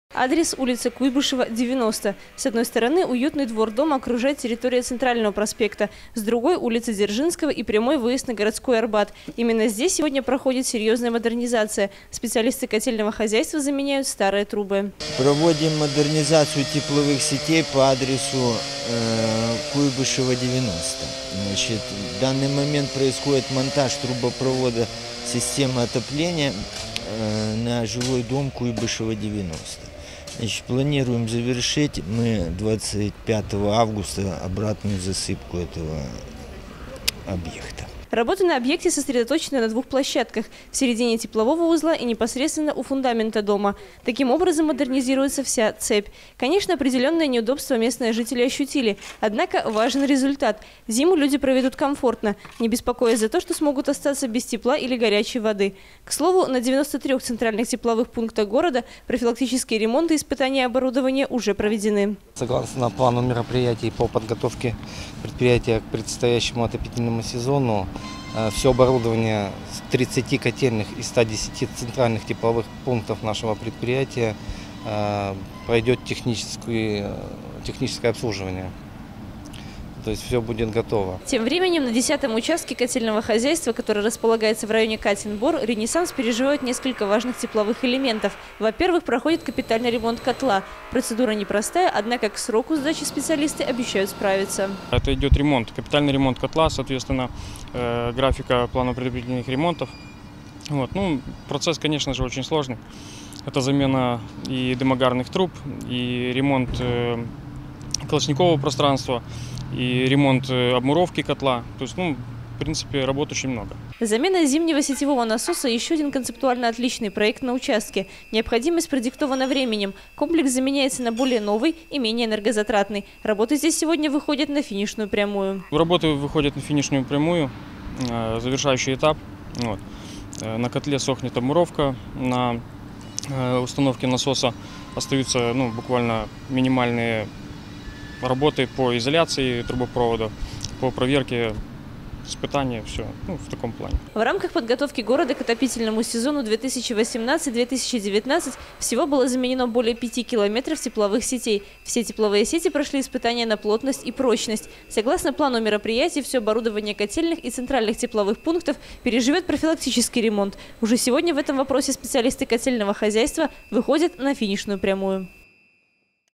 2018-09-04 г. Брест. Котельное хозяйство Новости на Буг-ТВ.
Журналист Анастасия Ноздрин-Плотницкая. Оператор Павел Малашенок
Несмотря на довольно высокие температурные показатели за окном, специалисты котельного хозяйства активно трудятся в рамках подготовки к отопительному сезону, причем большой фронт работ уже проделан. Все для того, чтобы включение систем теплопотребления прошло по графику. Наш корреспондент Анастасия Ноздрин – Плотницкая побывала на главных объектах города, где сегодня сконцентрирована деятельность представителей котельного хозяйства, и узнала, насколько готов областной центр к встрече с холодами.